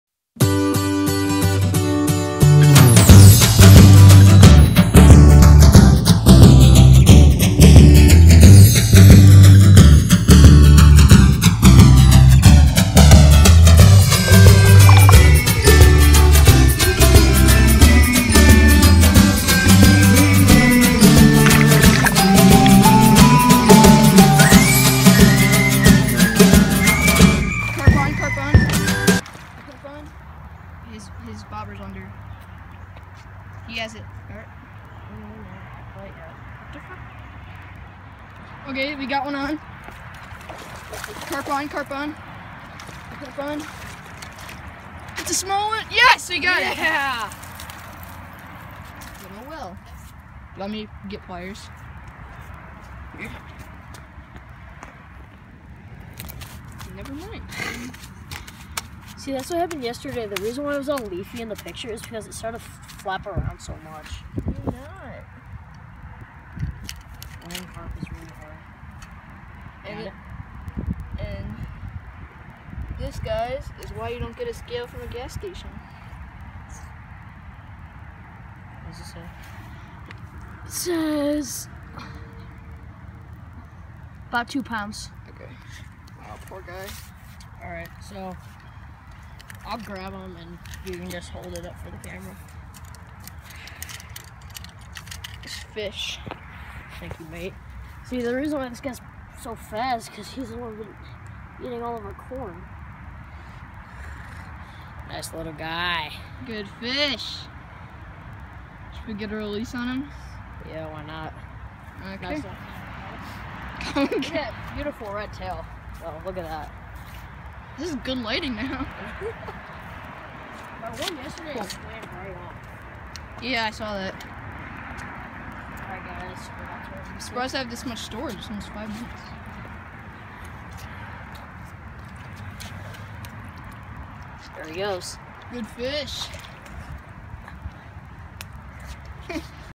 Carp Fishing at Herrick Lake Forrest Preserve | 11/15/15
Sorry for the lack of videos, my gopro has a piece chipped off and I cannot find the micro sd card. :/ They should be more soon and along with next spring!